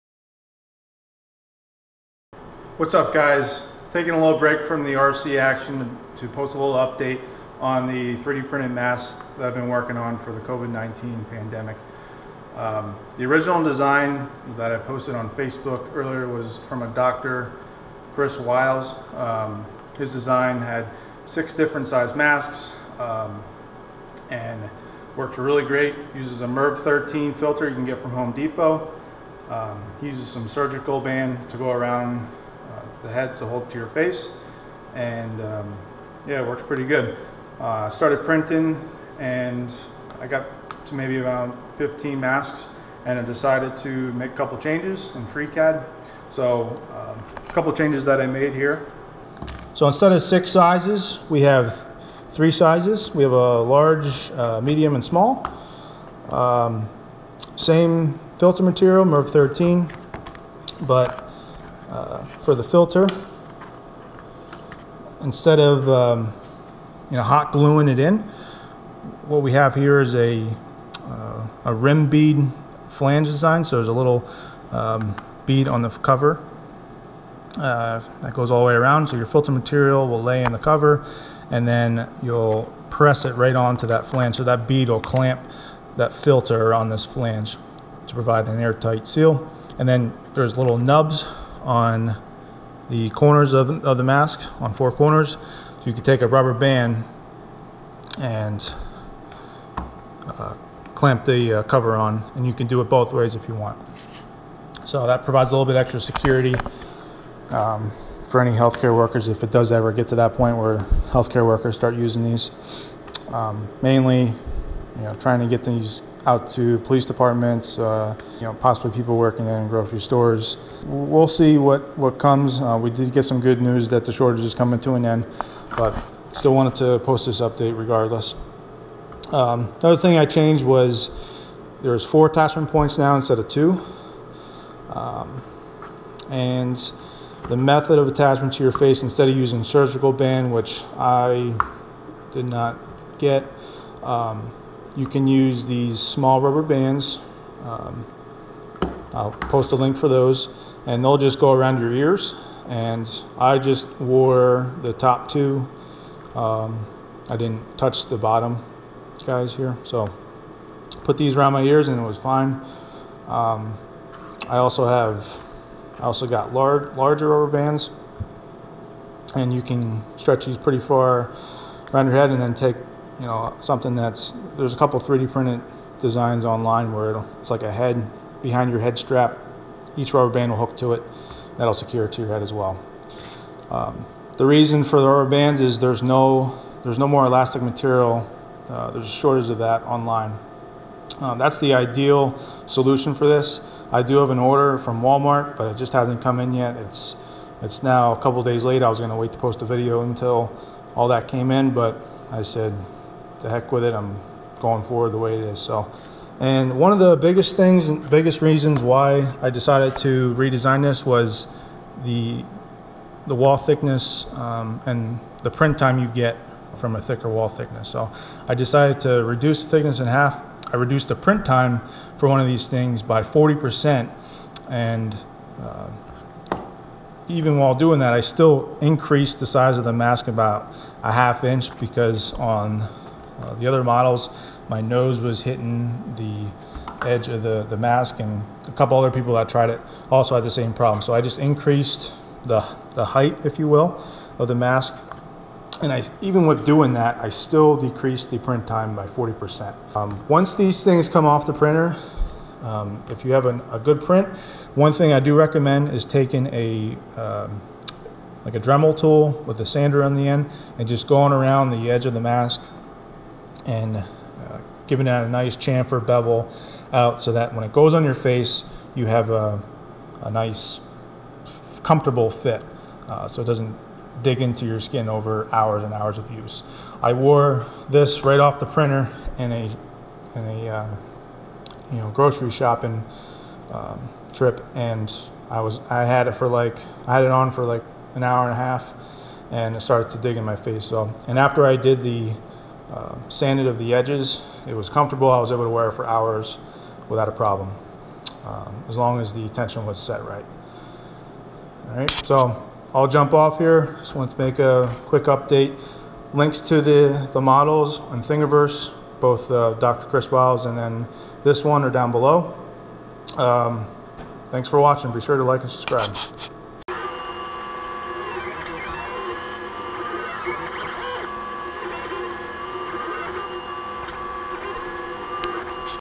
COVID 19 Masks N95 Alternative
COVID-19 Mask (N95 Equivalent)

(Dr. Wiles posting) (Version 2)

This is a remix from Dr. C. Wiles
-Streamlined design
-Single size cover for all sizes
-Rubber bands stubs on four corners of mask for extra security
-Filter clamped by cover with rim bead and flange design
-3 sizes instead of 6 (large/medium/small)
-Half the wall thickness for flexibility and 40% reduction in print time(works good with nylon materials)

Filter material is any MERV 13 from the local home stores (Lowe's, HD).
Elastic material is in shortage now so rubber bands around ears and/or printed rear head straps with rubber band connecting hooks will work

I have also posted the FreeCAD files so feel free to change the curve fit of the mask around the nose, cheek, etc. for anyone who's face these sizes do not fit

Thanks to Nick & B.Maroun for helping with the covers and design changes